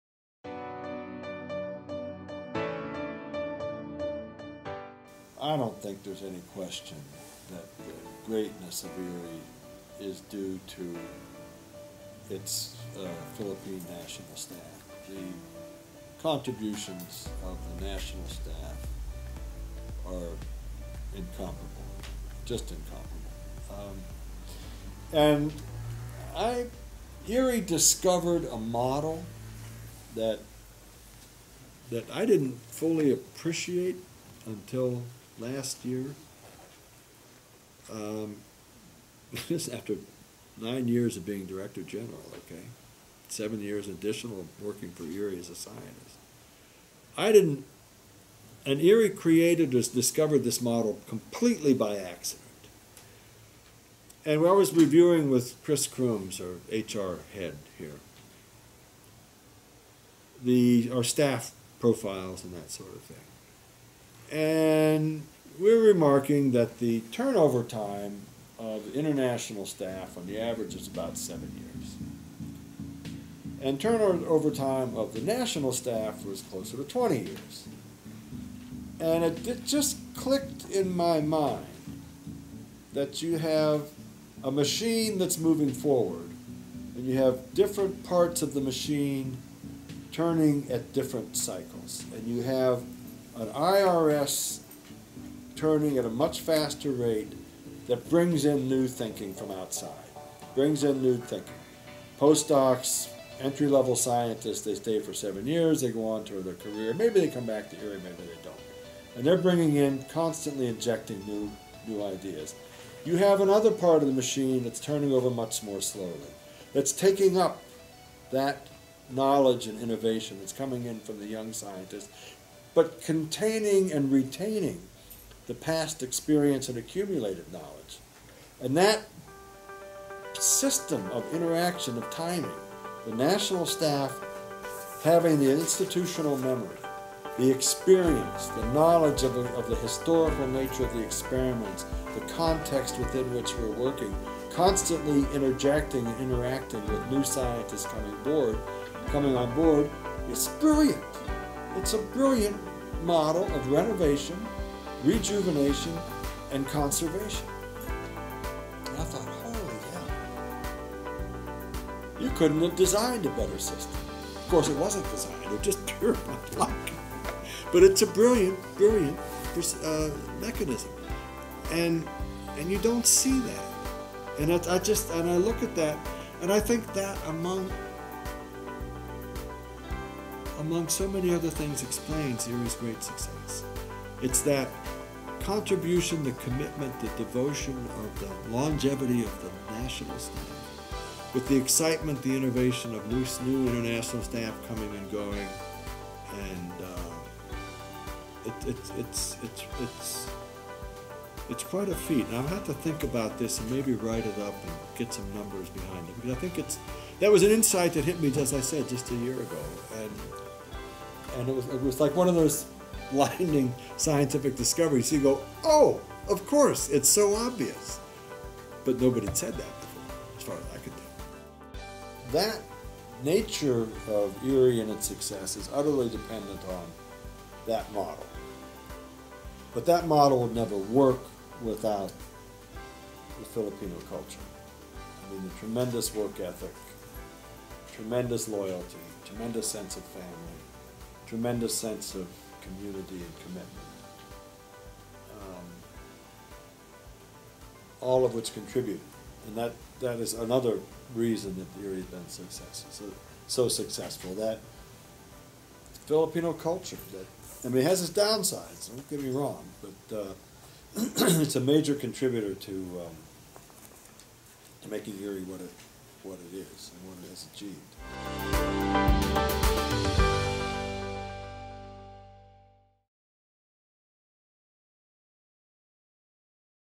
Pioneer Interview: Wheels of a brilliant machine--IRRI staff
Also see "An introverted :mad" scientist speaks bluntly and makes no apologies

Dr. Robert S. "Bob" Zeigler, an internationally respected plant pathologist with more than 30 years of experience in agricultural research in the developing world, has been the director general of the International Rice Research Institute (IRRI; http//irri.org) for the last 10+ years—the second longest tenure for an IRRI DG after only the Institute’s founding DG, Robert F. Chandler, Jr. (1960-72).

As DG, Bob set the Institute’s strategic direction and has also been a passionate spokesperson on a wide range of issues that affect rice growers and consumers worldwide. He retires in mid-December 2015.  Proclaiming himself an introvert, he gave an  IRRI pioneer interview, conducted on 28 August 2015, with predictable wit and candor. He covers his life before and throughout his professional life, which has spanned time spent in Africa, Latin America, Asia, and the U.S.

In this excerpt from the interview, he talks about the Institute's national and international staff as being the wheels of a brilliant machine.

"I don’t think there is any question that the greatness of IRRI is due to its Philippine national staff (NRS). The contributions of the NRS are incomparable, just incomparable. IRRI discovered a model that I didn’t fully appreciate until last year (after 9 years of being DG and 7 years additional as scientist in the past). IRRI created and discovered this model completely by accident. I was reviewing with Christine Croombes  [director of IRRI’s Human Resource Services] our staff profiles. We noticed that the turnover time of international staff (IRS), on the average, is about 7 years and turnover time of the NRS is closer to 20 years.

It just clicked in my mind that we have a machine that is moving forward with different parts turning at different speeds in that we have IRS turning at a much faster rate, bringing in new thinking from outside via postdocs and entry-level scientists. They come for 7 years or so and move on—maybe they come back later, maybe they don’t. They come in, constantly injecting new ideas.

We have another part of the machine that is turning over much more slowly. It’s taking up that knowledge and innovation that is coming in from the young international scientists, but containing and retaining the past experience and accumulated knowledge. That system—of interaction and timing involving the NRS having the institutional memory, the experience, the knowledge of the historical nature of an experiment, and the context within which we are working and constantly interjecting and interacting with new scientists coming onboard—is a brilliant model of renovation, rejuvenation, and conservation. You couldn’t have designed a better system.  But it wasn’t designed; it was pure luck. But it is a brilliant, brilliant mechanism. I think among so many other things, this explains much of IRRI’s great success. It’s that contribution, the commitment, the devotion, and the longevity of the NRS with the excitement and innovation of new IRS coming and going. It’s quite a feat and I’ll have to think about it some more and get some numbers behind it and maybe write it up.

As I said, this is an insight that hit me just a year ago. It’s like one of those blinding scientific discoveries—you go ohhh, of course, it is so obvious, but know nobody has said that before. That nature of IRRI and its success are utterly dependent on that model.  But that model would never work without the Filipino culture, its tremendous work ethic, tremendous loyalty, tremendous sense of family, and tremendous sense of community and commitment."